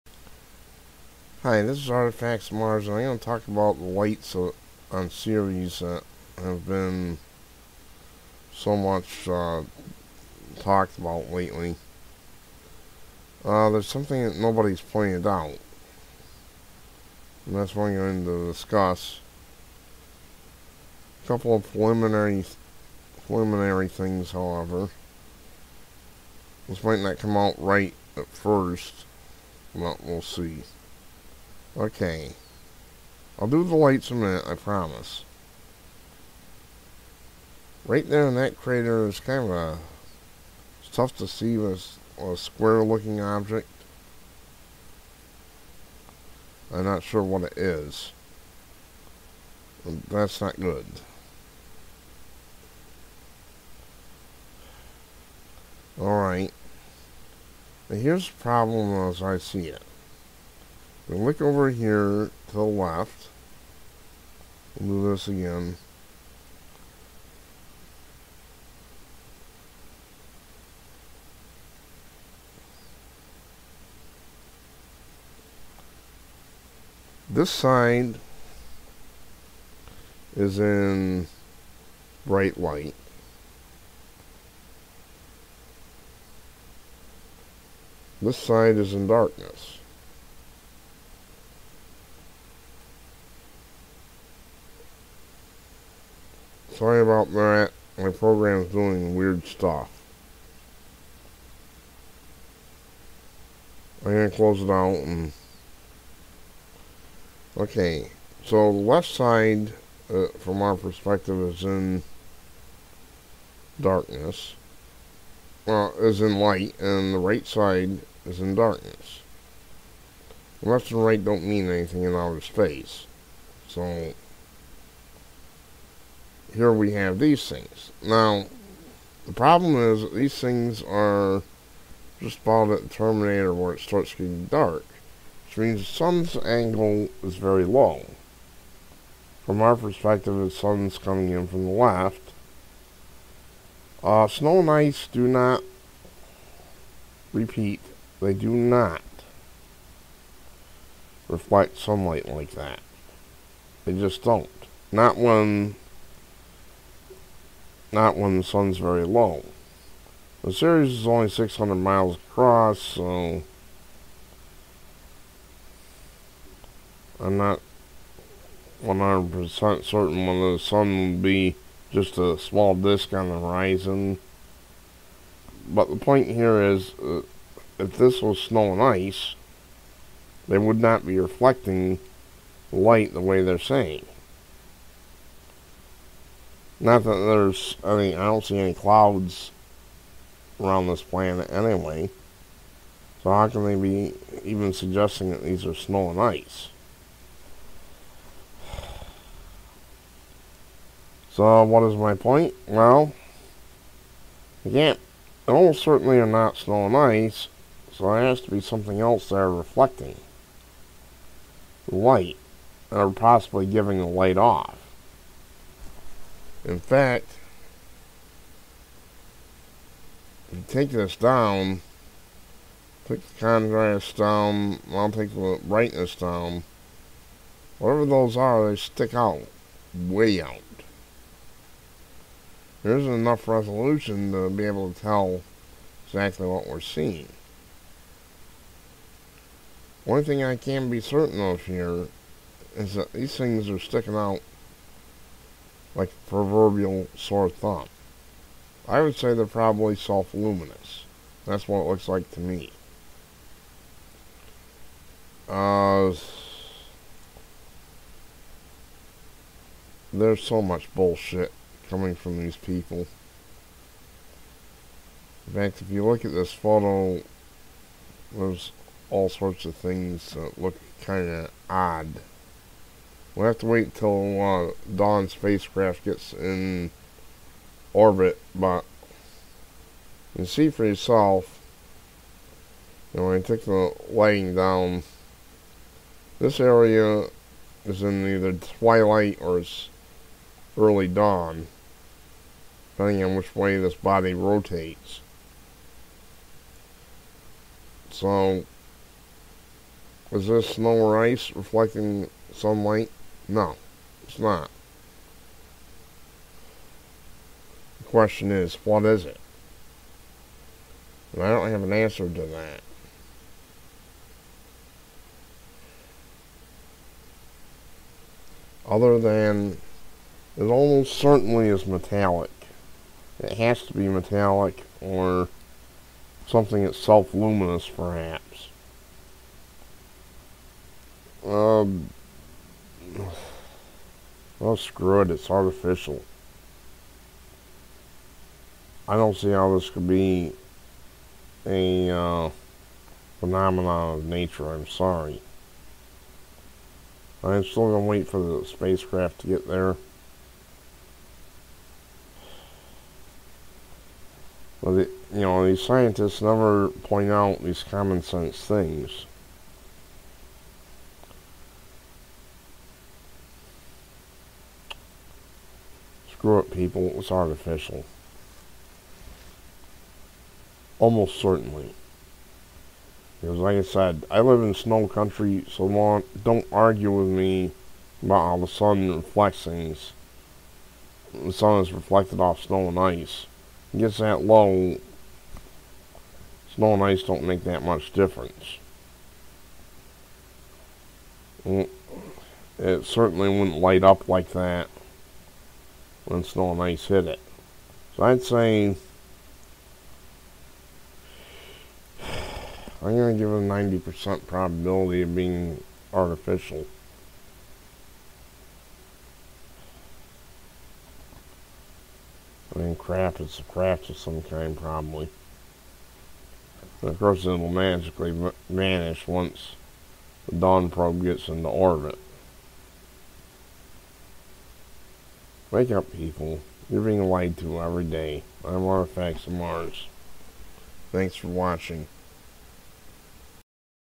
Lights on Ceres are almost certainly artificial!
The photo showing what appears to be lights on Ceres indirectly proves that they are artificial because the sun's angle is too low, and these are too bright, for these to be reflections of any kind of ice or snow. They are almost certainly metallic.